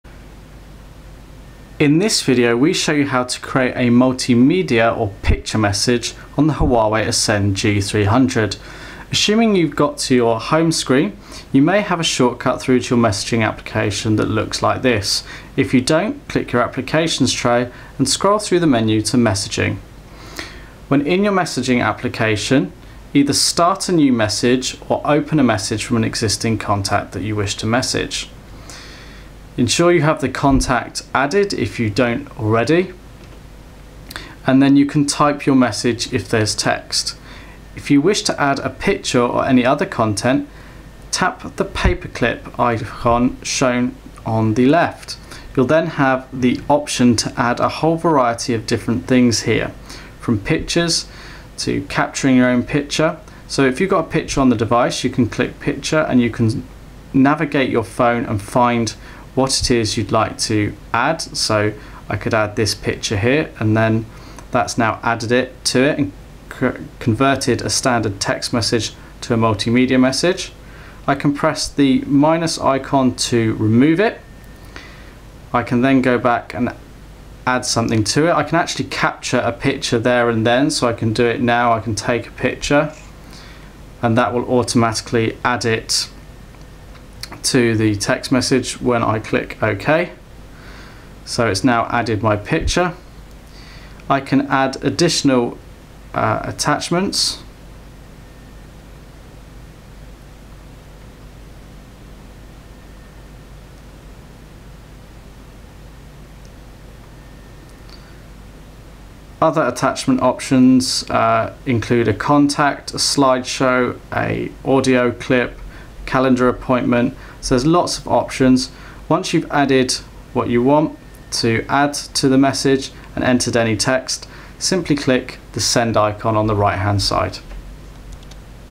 How To Create Picture/Multimedia Message - Huawei Ascend G300
A video how to, tutorial, guide on creating a picture or multimedia message on the Huawei Ascend G300.